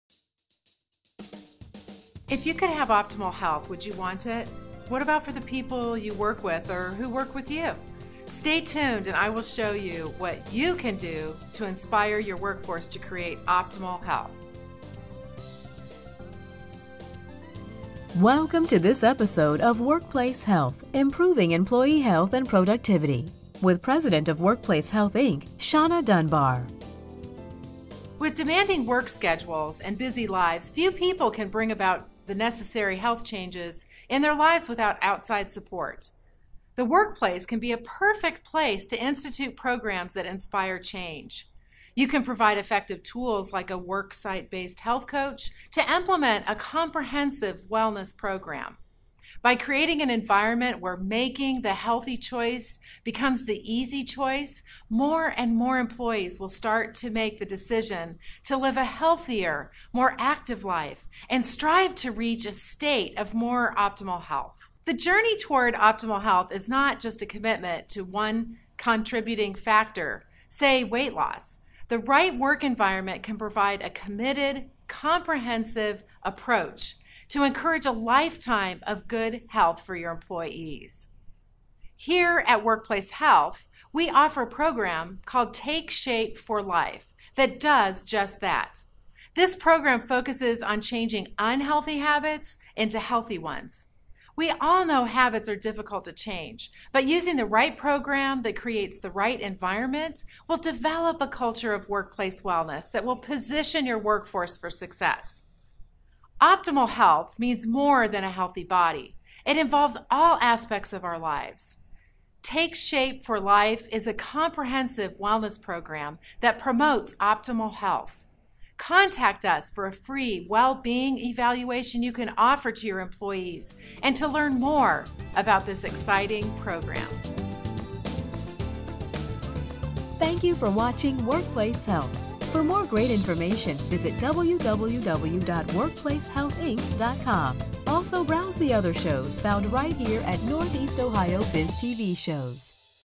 Inspiring Optimal Health
Shanna Dunbar shares how to inspire optimal health in the workplace